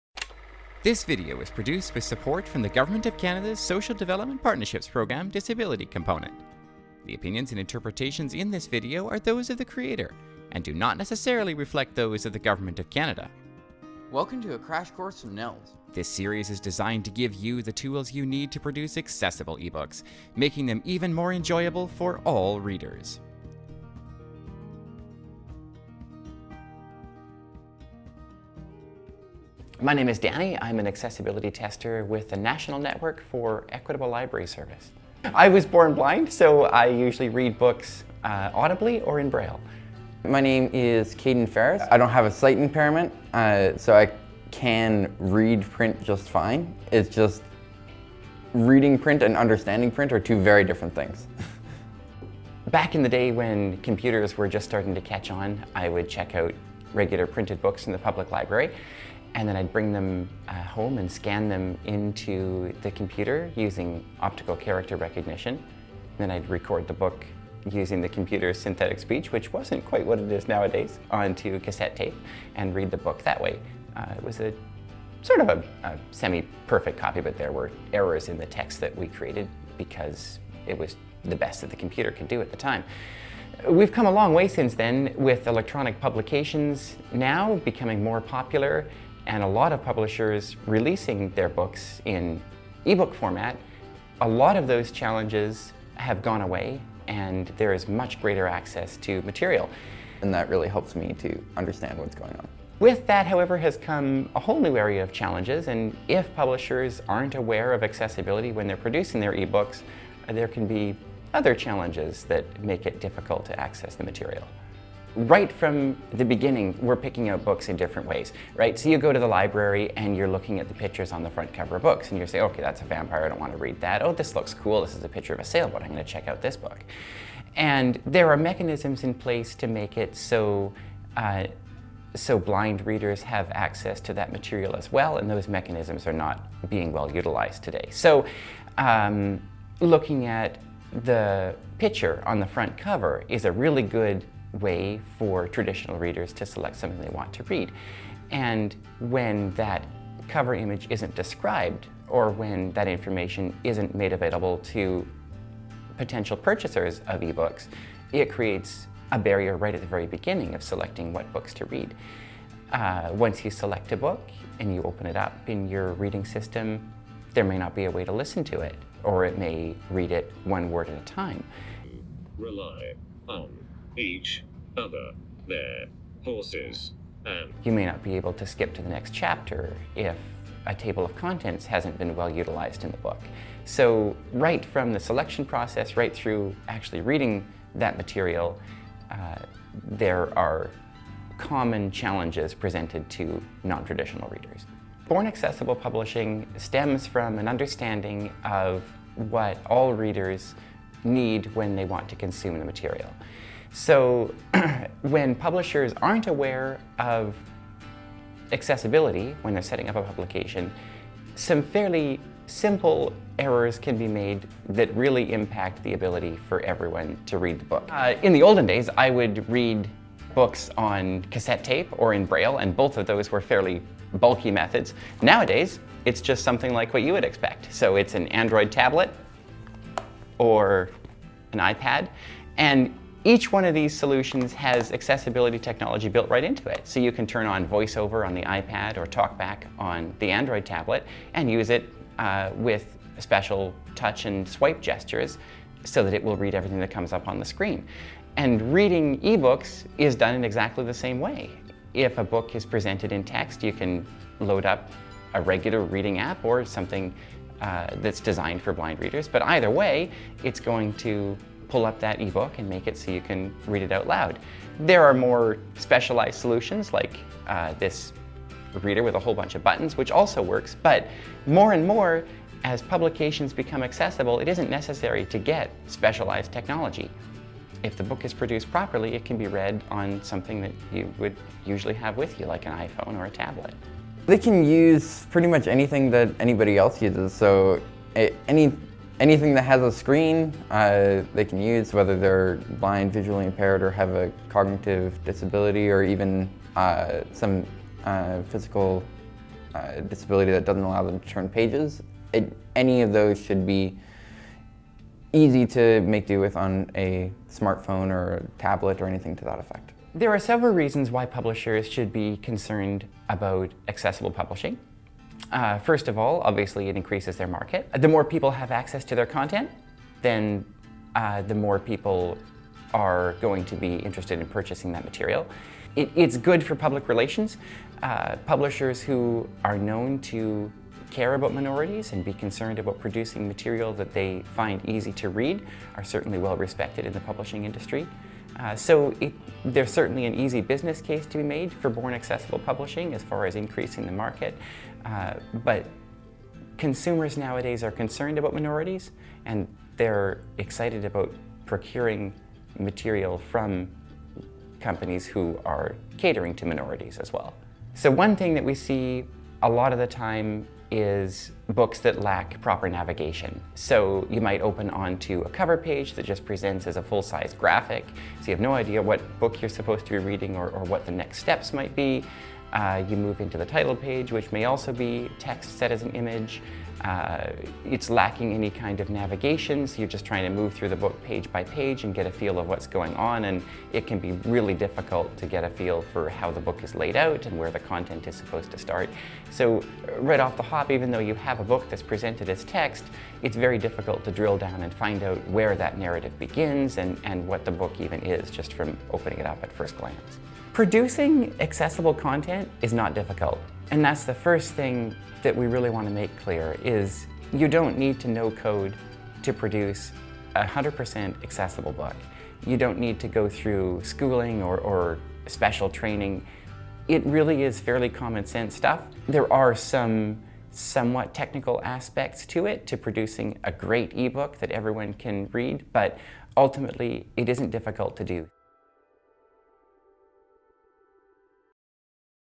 A Crash Course in Ebook Accessibility Introduction: The Benefits of EPUB
It's easy to produce accessible content!
This video series will go over some of the key components of making accessible ebooks.
Accessible publishing improves the look, feel, and navigation of your book, and born accessible publishing just makes good business sense!
Learn more about publishing accessibly, here, in NNELS' Accessible Publishing Best Practices: Guidelines for Common EPUB Issues in Plain Language